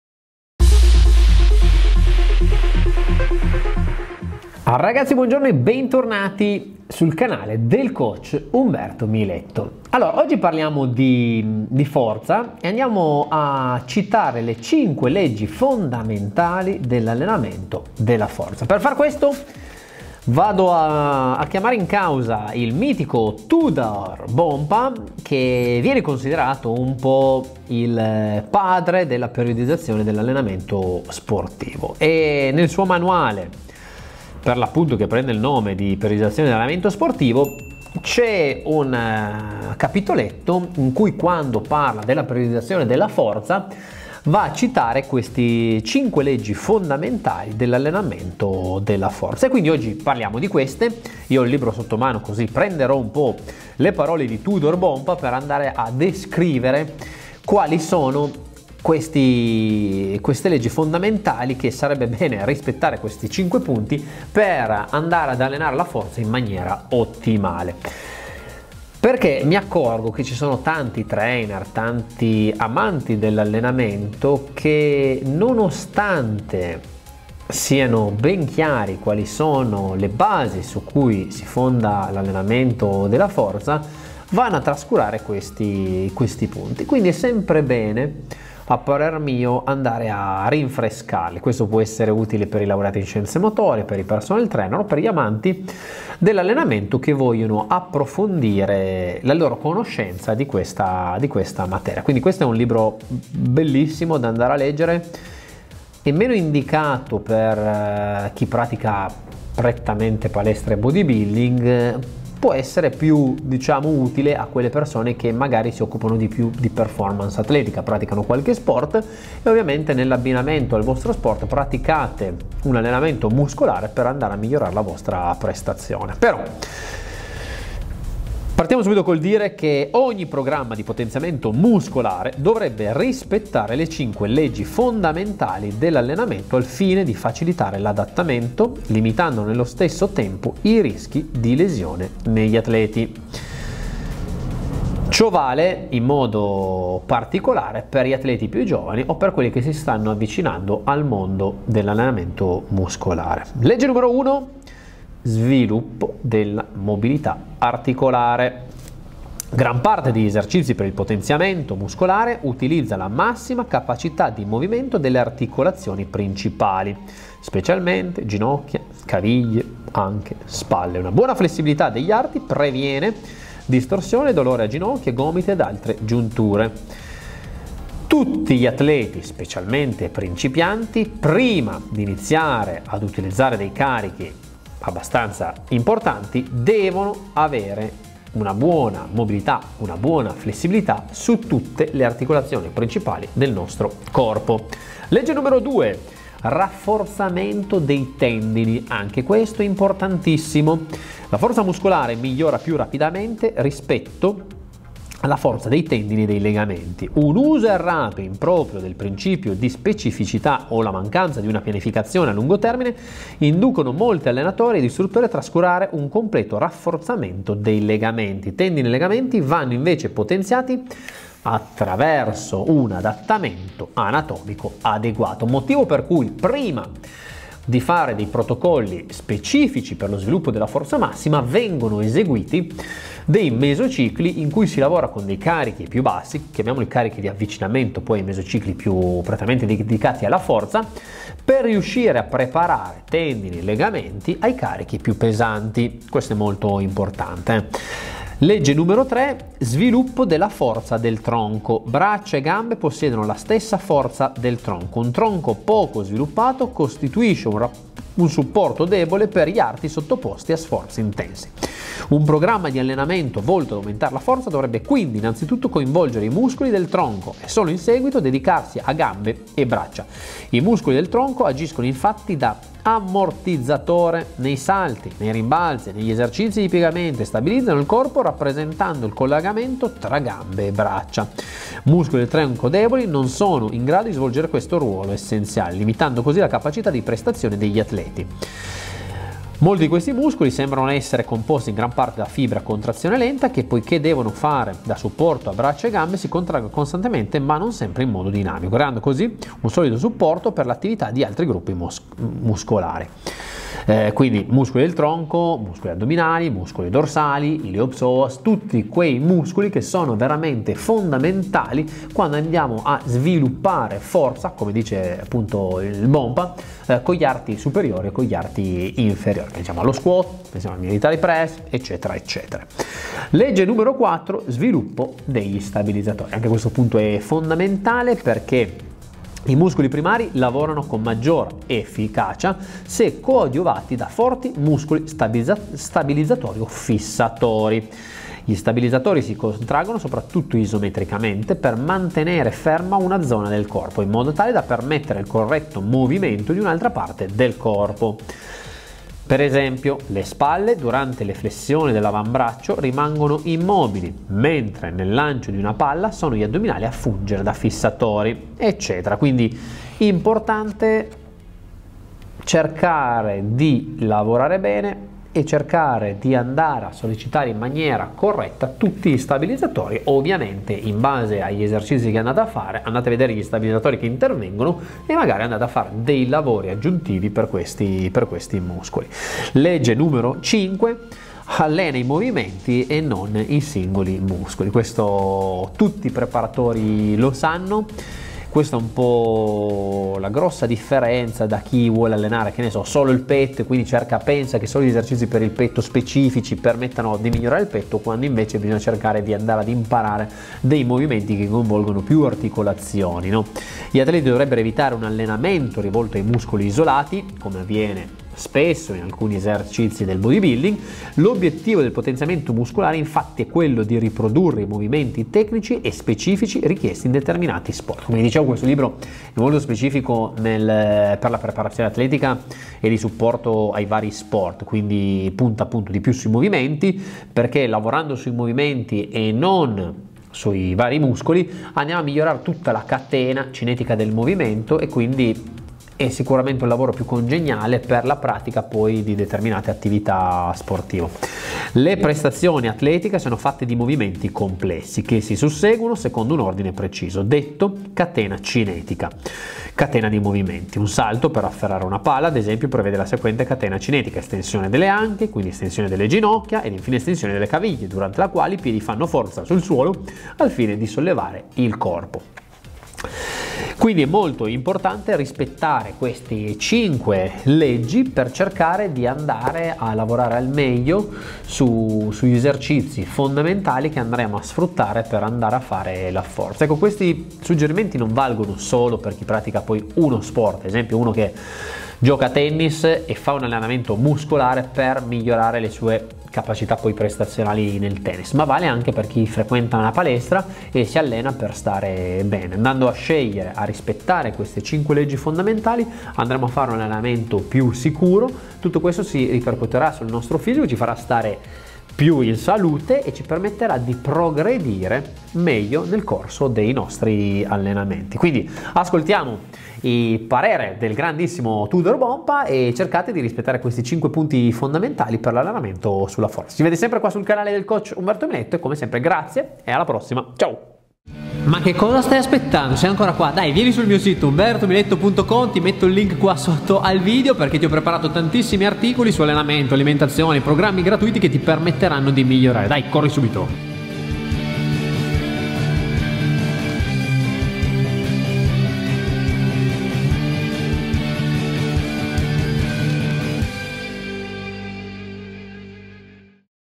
Le 5 leggi fondamentali per allenare la forza
In questo video vediamo le 5 leggi della Forza formulate da Tudor Bompa 📖

Accedi immediatamente ai miei Sistemi d'allenamento. Scarica i video d'allenamento gratuiti ➡️